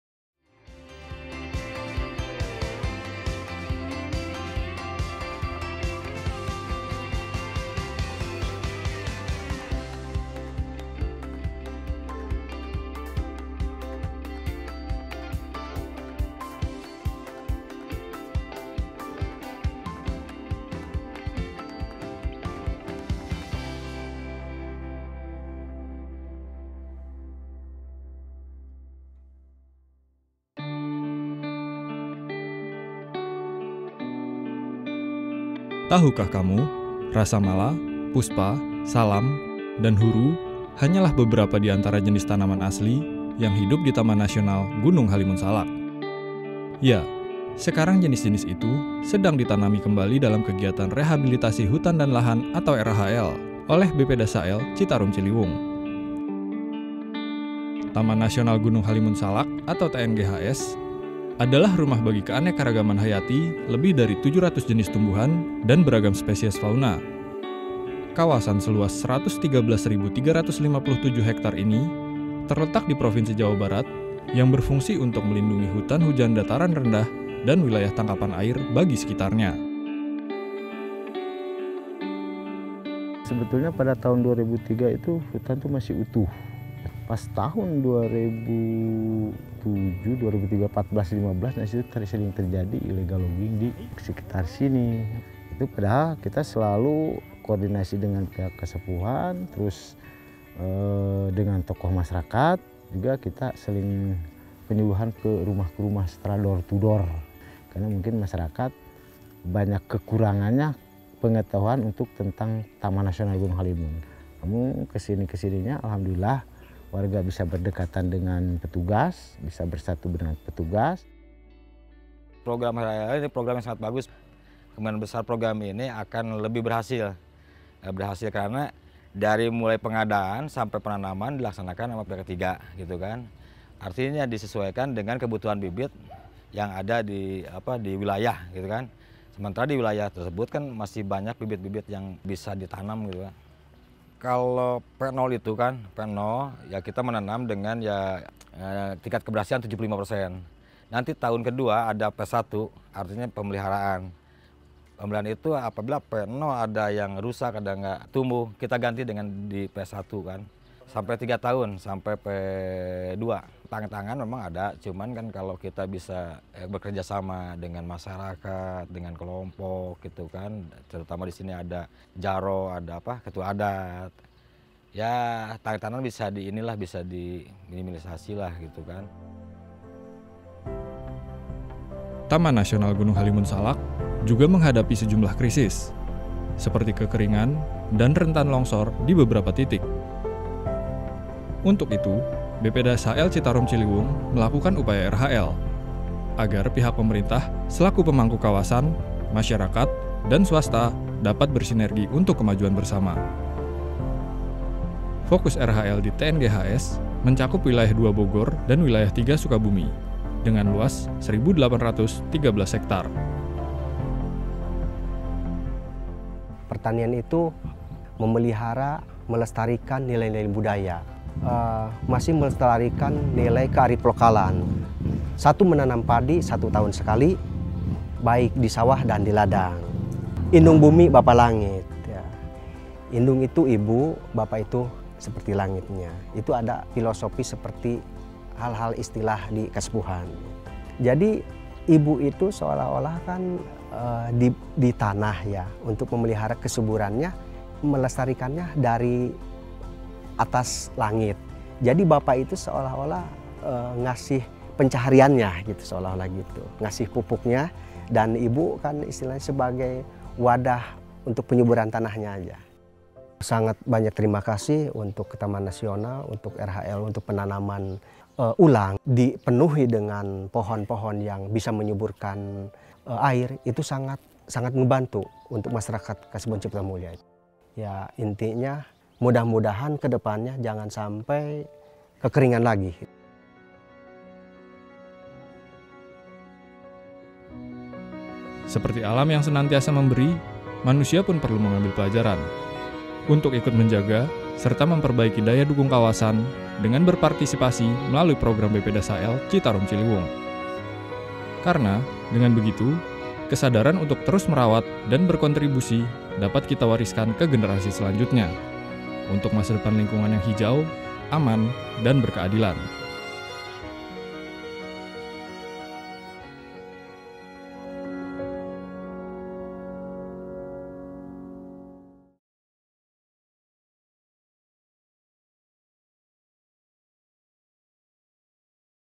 Mengembalikan Kehidupan Taman Nasional Gunung Halimun Salak
Hallo sobathijau, dalam upaya pemulihan lahan kritis terutama yang terdapat dikawasan hutan konservasi, yang mana kawasan hutan memiliki fungsi pokok sebagai perlindungan sistem penyangga kehidupan, untuk itu BPDASHL Citarum Ciliwung di tahun 2019 melaksanakan kegiatan Rehabilitasi Hutan dan Lahan (RHL) salah satunya di kawasan Taman Nasional Gunung Halimun Salak.
Lokasi yang di tanami di Wilayah  TNGHS yaitu blok Cikaniki, blok Gn Butak, blok Gn Cikaniki, blok Gn Kencana, blok Gn Salak II, blok Gn Talaga, blok Gn Bodas, dan blok Gn Kendeng dengan luasan total yaitu 1.813,30 Hektar.

Dalam Pelaksanaan kegiatan ini melibatkan berbagai pihak dan juga pendampingan secara multisektoral antara BPDASHL Citarum Ciliwung, Balai TNGHS, Masyarakat desa konservasi, tim pengawas dan penilai, tim pengendali, serta masyarakat desa konservasi.
Film by : BPDASHL Citarum Ciliwung dan Rekam Nusantara Foundation